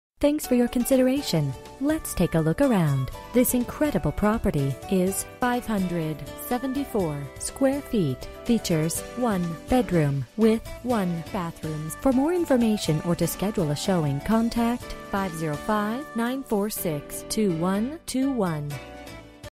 Residential for sale - 134 DURAN A, Santa Fe, NM 87501
Located on historic Duran Street, a few blocks from the downtown Plaza, this condo is a great find.  One bedroom and one bath near to all the amenities of downtown and tucked into a neighborhood that offers charm, privacy and convenience.  Presently used as a long-term rental.

Bedrooms: 1
Bathrooms: 1 bathroom
Square Feet: 574
Price: $245,000

For more information about this property, please contact Tai Bixby & Associates at or . You can also text 3336445 to 67299.

02/13/2016 03:46:01 am

Last modified: